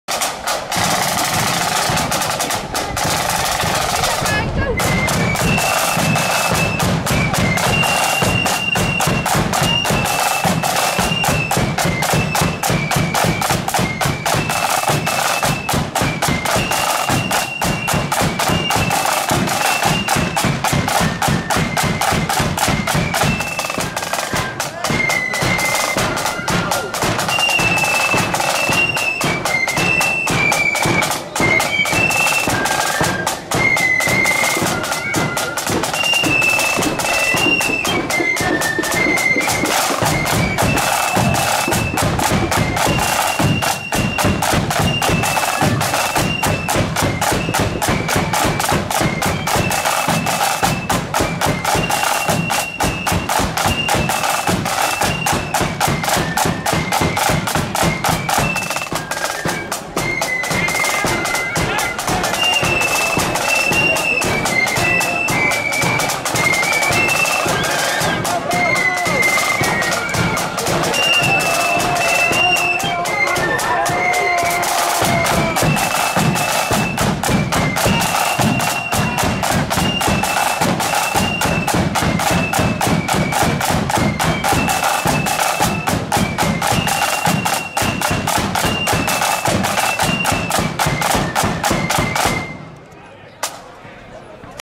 Upper Falls Protestant Boys (No.2) @ S.B.Y.C.'s Parade 31/07/21 (4K)
Upper Falls Protestant Boys Flute Band parading through the streets of South Belfast at South Belfast Young Conquerors Flute Band's annual parade held in Belfast on Saturday 31/07/21.